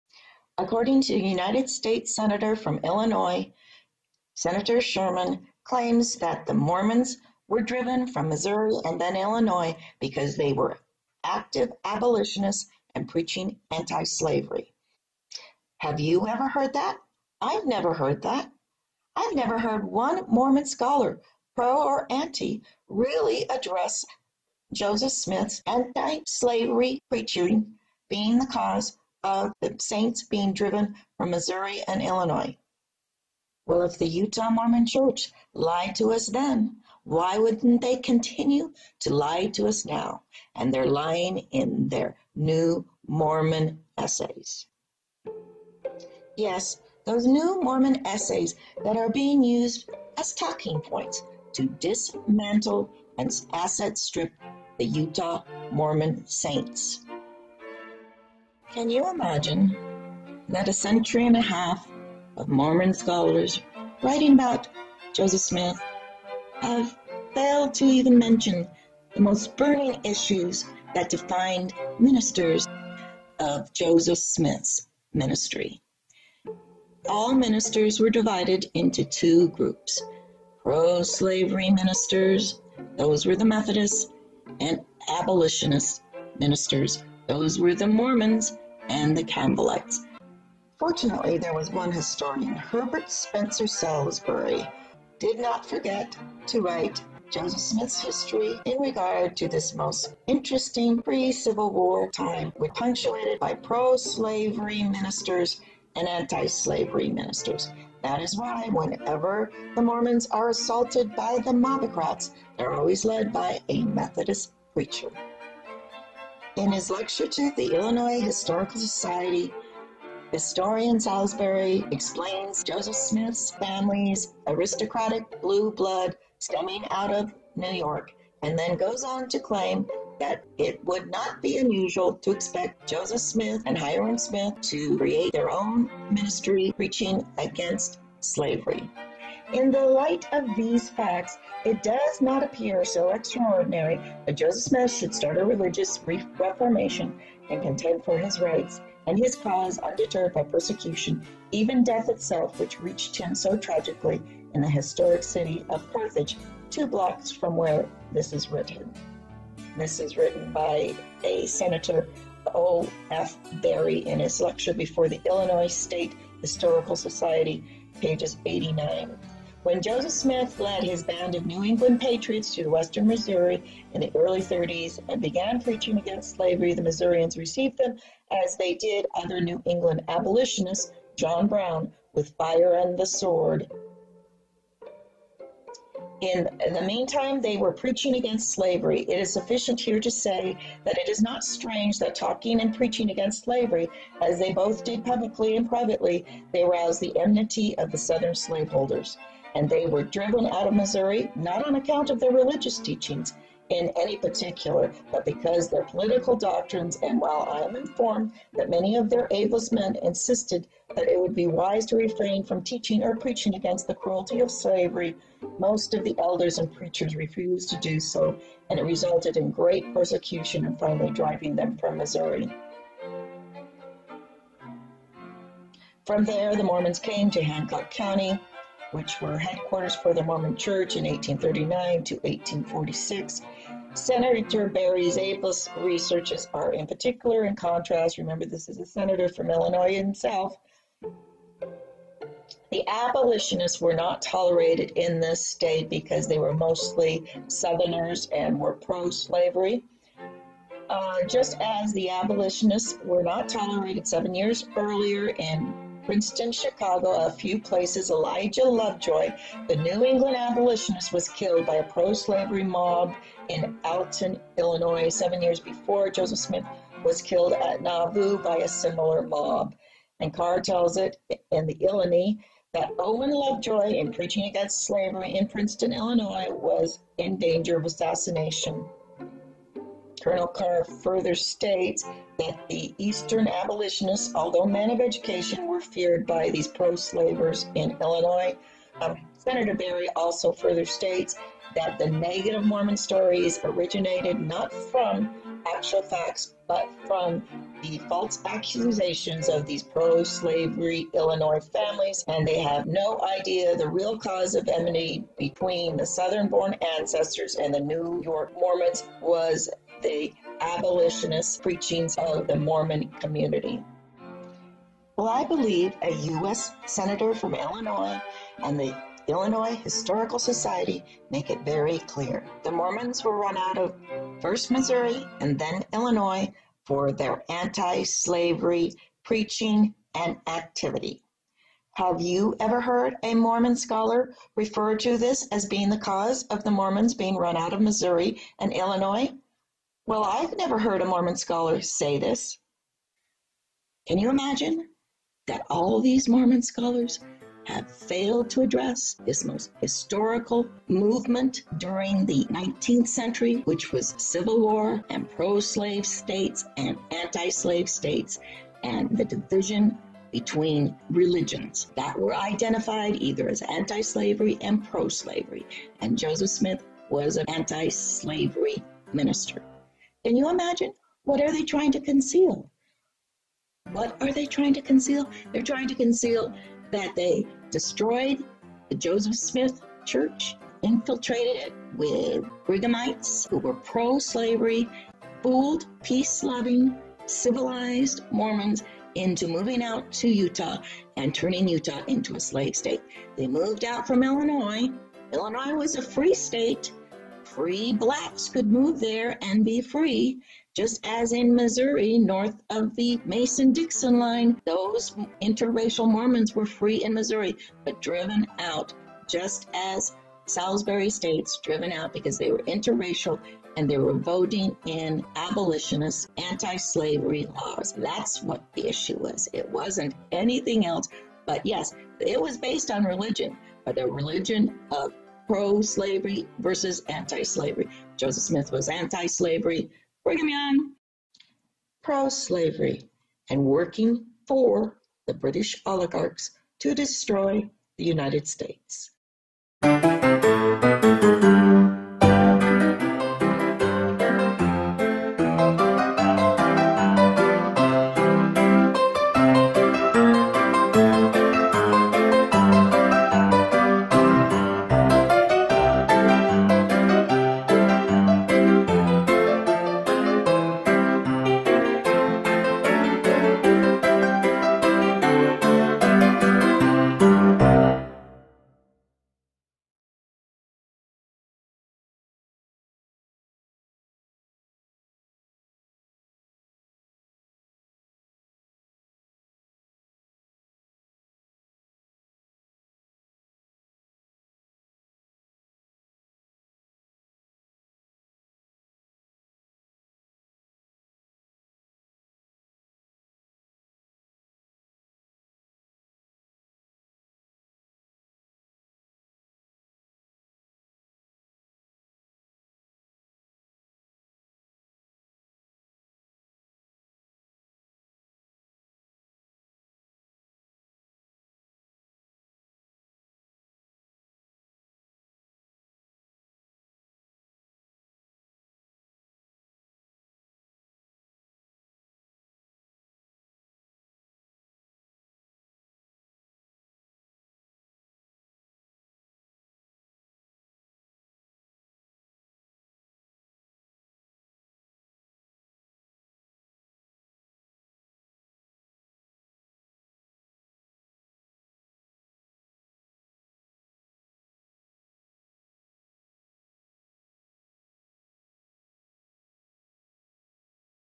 JOSEPH SMITH KILLED FOR BEING ANTI-SLAVERY: SCHOLARS LYING TO ASSET STRIP LDS CHURCH FROM INSIDE
Why do Mormon scholars debate endlessly polygamy, money digging, treasure hunting of Joseph Smith. Because they are concealing the truth. The Mormon movement was part of the active 19th century Abolitionist movement and Joseph Smith was killed for preaching against slavery, and for no other reason. Just like preachers Lovejoy before him and John Brown after, Joseph Smith was killed for his antislavery activities, which included the anti slavery Mormon voting block on election day in Gallatan Missouri.
Brighamites were infiltrators and agent provocateurs paid by British Banking Oligarchy to destroy the Mormon Movement.
A US senator from Illinois  Lawrence E Sherman compared Smiths death to that of Elijah Lovejoy, another antislavery activist who was murdered by proslavery  Illinois masons five years before Smiths death. In fact a black antiSlavery Massachusetts Mormon, named himself Elijah Lovejoy after the antislavery Illinois martyr.  Smith was not an “abolitionist” which was a political movement advocating violence to stop slavery which resulted in the Civil War.  Smith clearly stated to Josiah Quincy and Charles Adams in April 1844 he was antiSlavery, soundly against the Missouri compromise as being a submission to proslavery factions. Smith proposed freeing all slaves by selling US land and compensating slaveholders for freeing their slaves just as Britain did when it ended slavery.  Nauvoo was a port on the underground railroad as was Quincy and Keokuk, where my Mormon ancestors participated in the antiSlavery movement. The Underground Railroad ended North in Ohio where many Mormons participated in Legal battles to free slave and fight against the Fugitive Slave Laws which were enslaving free blacks through kidnap and human trafficking. All the violence at the Missouri voting poles concerned the Mormon antiSlavery vote. At the time Missouri outlaw even free blacks from settling in its territory and the Mormons were going to vote that illegitimate legal prohibition out of law. Polygamy was simply female slavery pure and simple and Smith fought this form of slavery till his last breath, telling President Marks the secret practice polluting Nauvoo must be rooted out and destroyed.  No politician at the time believed Smith introduced polygamy, including Mark Twain who mentions in his Book Roughing it that Brigham Young was the architect of the fiendish practices. Bennett's slanderous book accusing Smith of polygamy was taken as a joke with Bennett having no credibility except for brain addled Lilburn Boggs, a proslavery border ruffian who was also chased out of the US along with his father in law, Davy Crockett, of the  Alamo debacle. Boggs was John D. Lee's first employer and sent John D. Lee to work with Brigham Young in overthrowing the US in favor of Britain. Why don't Mormons read John D. Lee who described Polygamy being introduced to him by Brigham Young and it wasn't Harem polygamy but a Freelove Wife Swapping system in which diaries were kept about when a man slept with a woman for only one or two nights so that nine months later a baby born of the one night stand could be traced to a bloodline of one of the Brighamite Libertines. This system of Wife Swapping was a common practice by many Free Thinking societies at the time such as the Oneidas and the Cochranites.  Years later Joseph Quincy commented that Smiths proposal to compensate slaveholders peacefully was a brilliant and far ahead of its time. You confused Smith, a brilliant religious leader who invited all interracial Americans to join his moments to free all people, with the traitorous Quorum of the 12. Are you aware of the Mack Family being wealthy landowners and bankers in Michigan. Are you aware that John Smith, Josephs great uncle was a Dartmouth scholar who introduced the ancient language curriculum, including Hebrew, Coptic, and Greek, into the U.S. Just Google it rather than continue the charade of Mormon historical fiction.  The Expositor was destroyed not because of old stale accusations of polygamy but because it was being used to incite Carthage mobs to invade Nauvoo and cause and insurrection. The City council meeting June 8 tells of a plot to kill the entire Hyrum Smith family, kidnap his eldest daughter into slavery, a contract killing being placed on Smith's head by William Law and the Expositor being used to counterfeit money on the New York bank. Smith told Quincy he was against the Mexican  American war because of its aspirations to turn the south and the west into slave territories, knowing it would hem in the US. Everything you think you know about Mormonism is a total fiction but most are too lazy to do original research.  Brigham Young was not a Joseph Smith Mormon, in fact Smith had only disdain for Young and knew the quorum was working against him. Not hard to figure out with the entire Quorum of 12 testifying against Smith in Missouri and trying to have him executed for treason